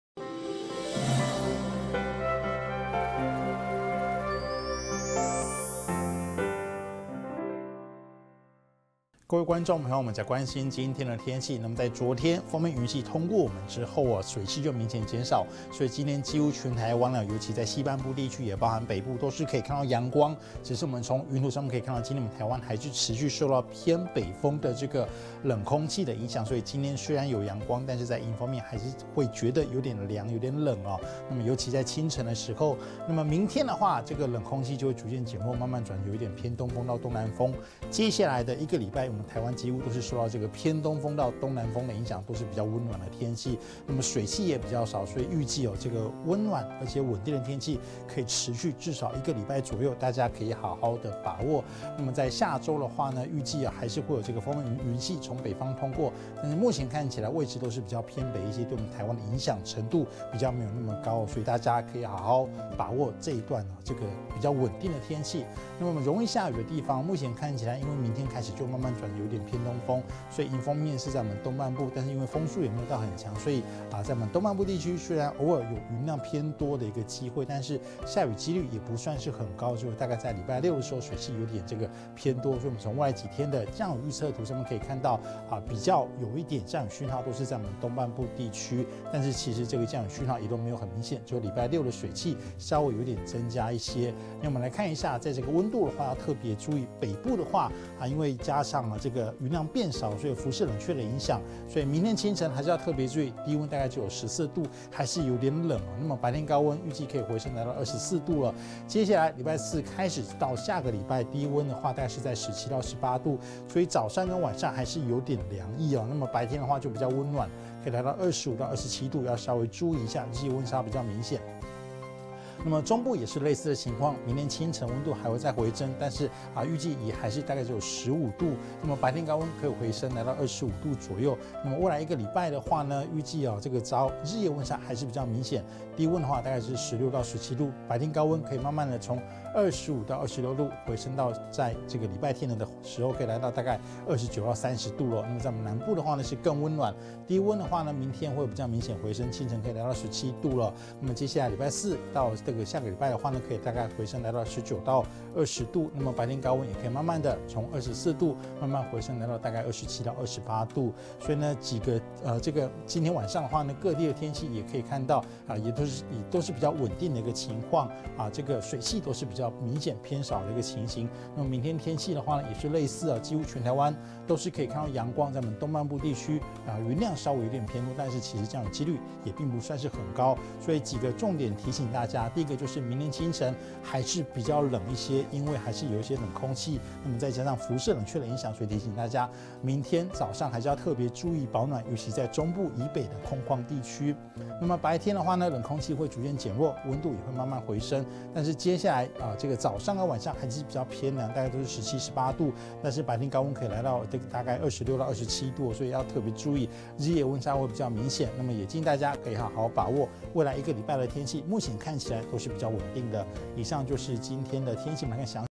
冷空氣持續減弱 日夜溫差大留意保暖
高壓東移 冷空氣減弱轉溫暖東風
輻射冷卻 周三清晨中部以北仍冷
清晨僅13度 早起添加保暖衣物
把握溫暖穩定天氣 持續至少一周